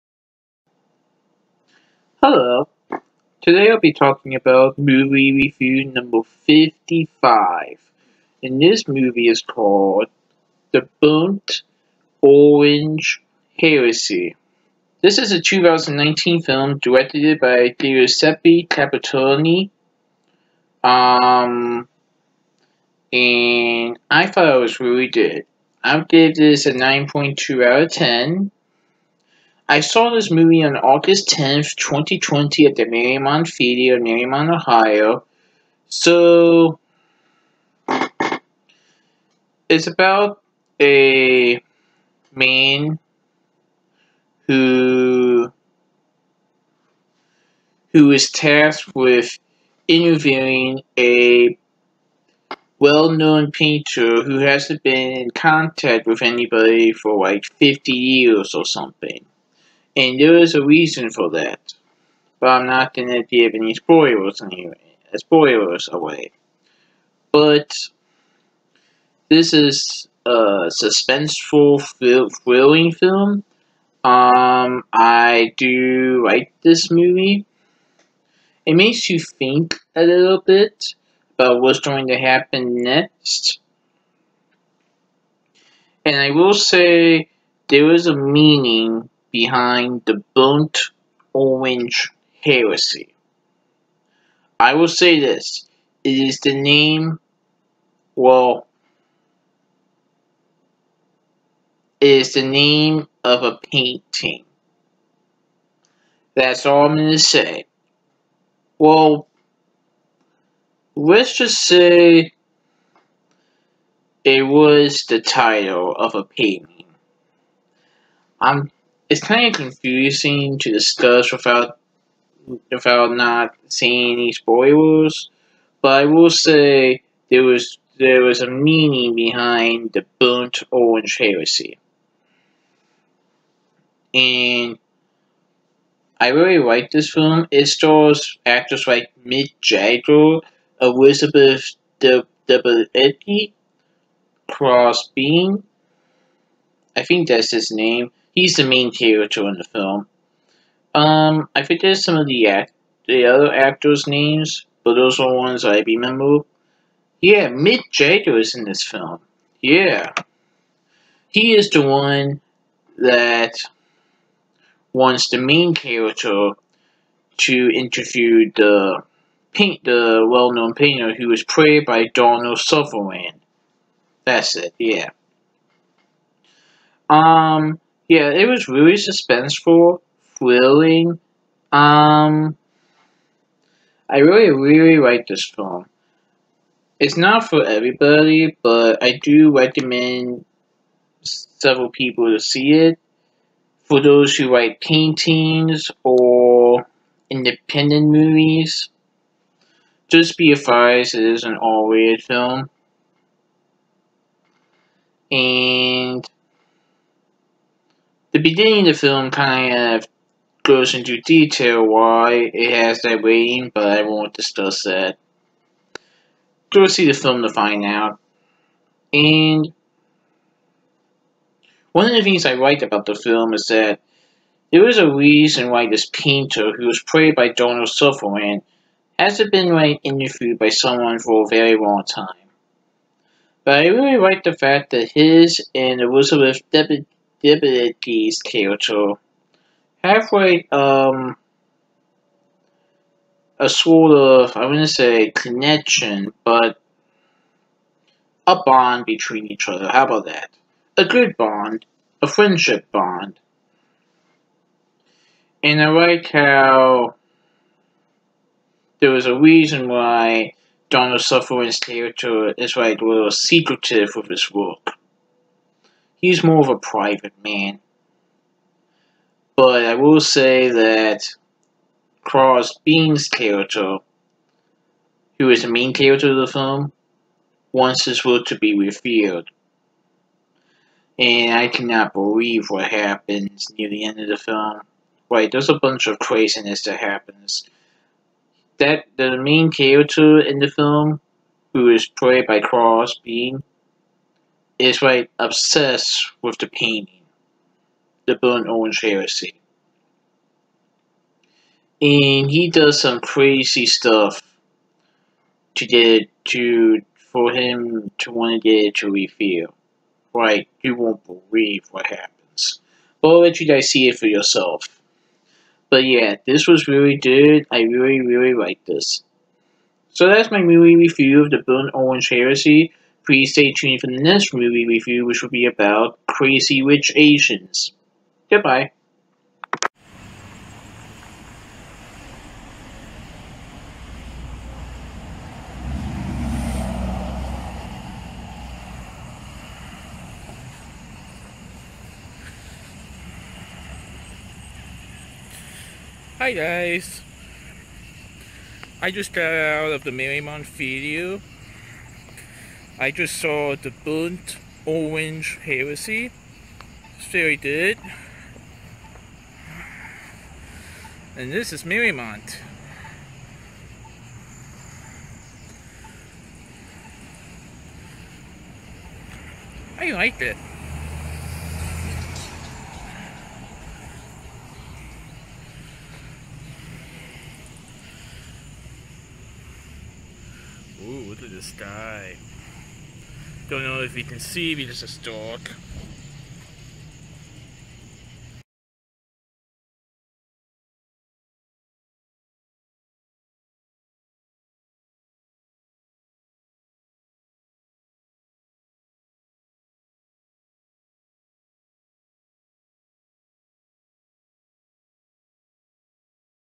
Movie Review #55: The Burnt Orange Heresy
I saw this movie on August 10th, 2020 at Mariemont Theatre in Mariemont Ohio in Auditorium 4. This is a 2019 movie distributed by Sony Pictures Classics and Ingenious Media.